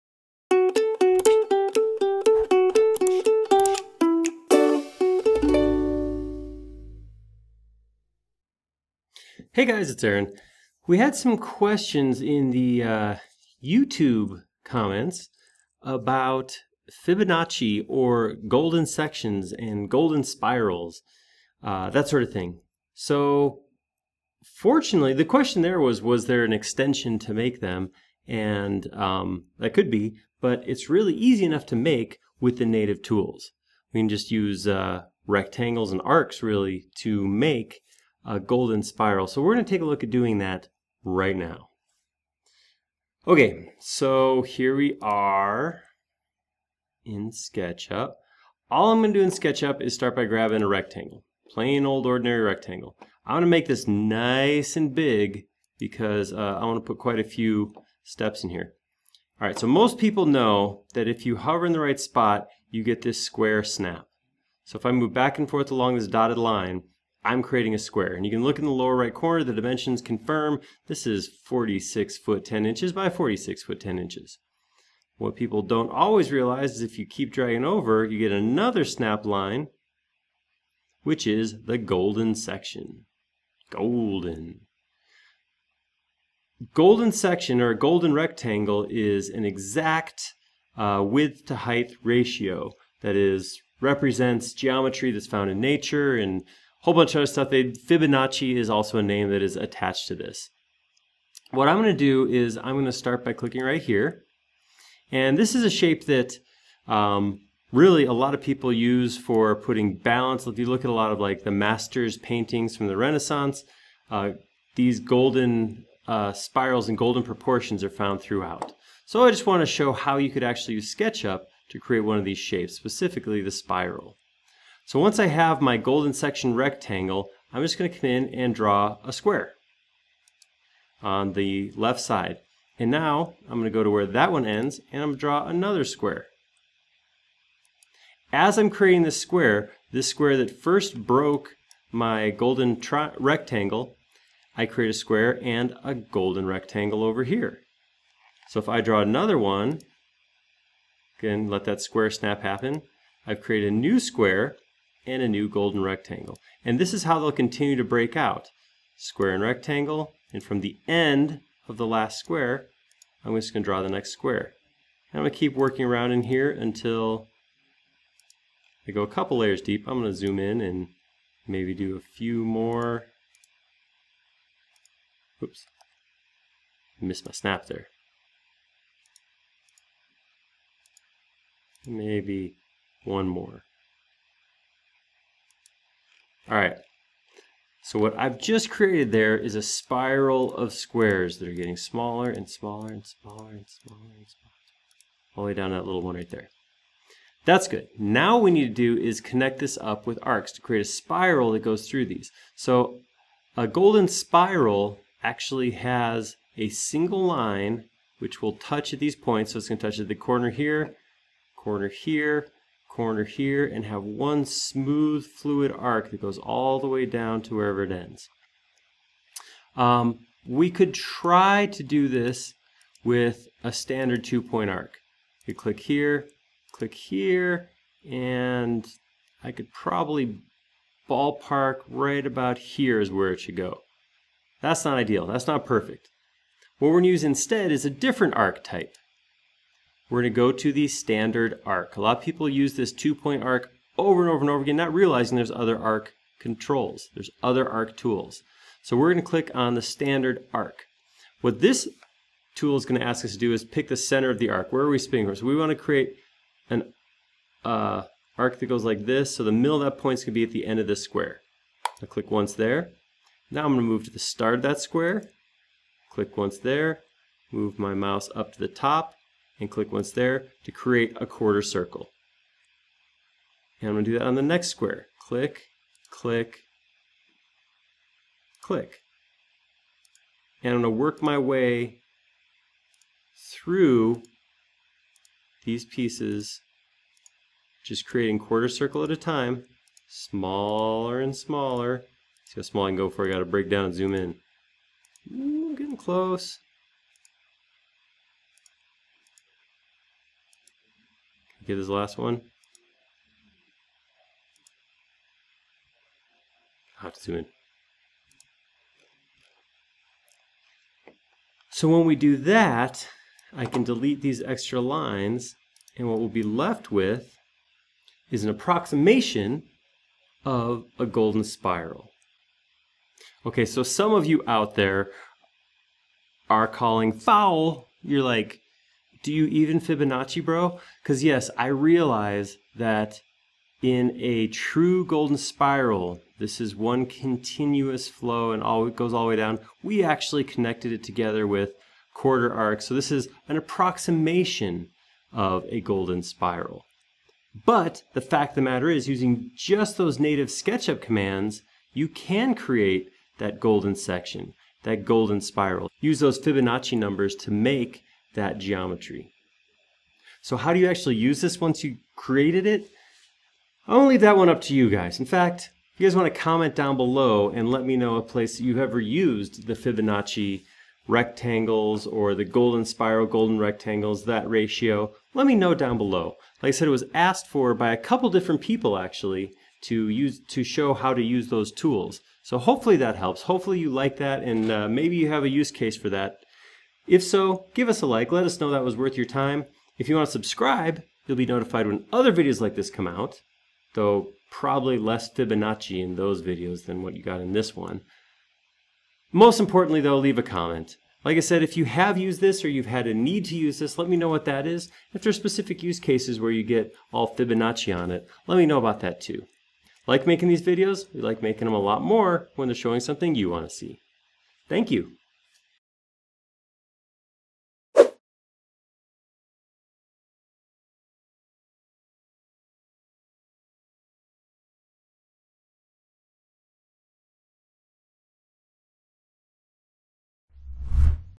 How to make a golden spiral in SketchUp - Skill Builder
Using inspiration from some SketchUpper questions, we wanted to show you how easy it is to make a golden spiral using only native tools in SketchUp. No extensions required! Follow along as we use the Arc and Rectangle tools to create a golden spiral.